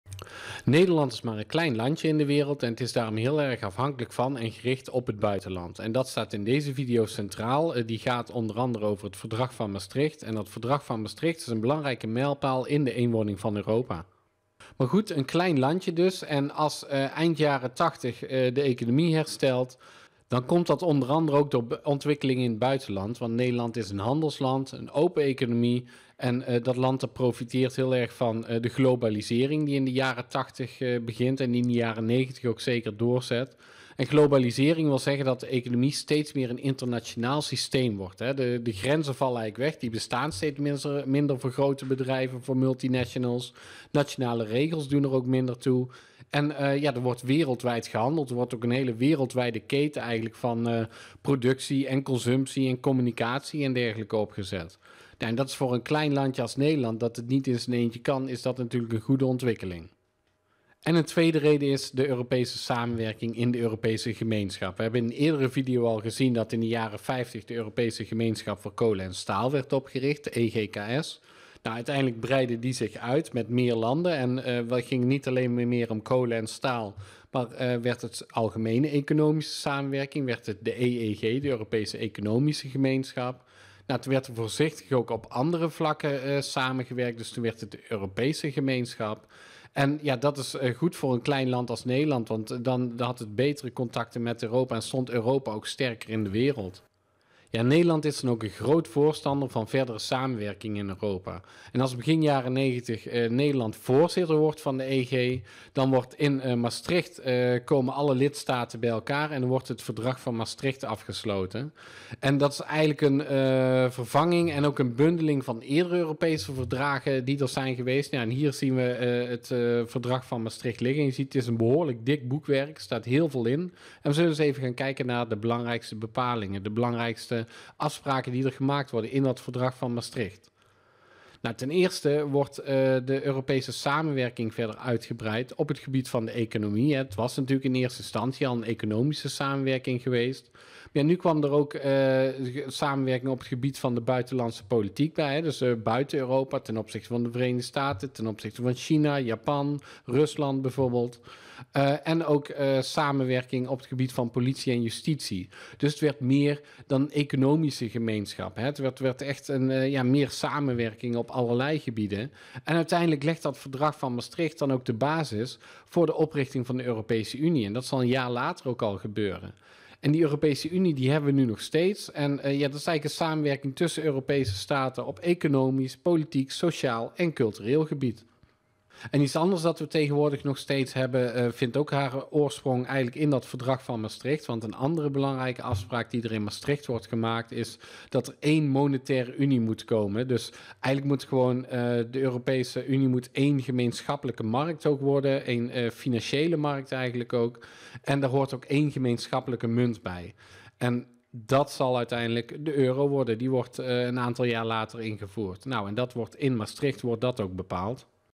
13. Het Verdrag van Maastricht (havo HC Nederland)
Deze video is voor de bovenbouw van de havo. Hij hoort bij de historische context ‘Nederland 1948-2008'.

Uit de stofomschrijving:
Aan het einde van de jaren 1980 herstelde de economie zich. Als handelsland profiteerde Nederland van de Europese samenwerking en de globalisering. Nederland was voorstander van verdere samenwerking in Europa. Afgesloten onder Nederlands voorzitterschap, legde het Verdrag van Maastricht de basis voor de Europese Unie en de invoering van de euro. Tot 2008 kende Nederland een grote economische bloei, maar ook een toename van de sociale verschillen.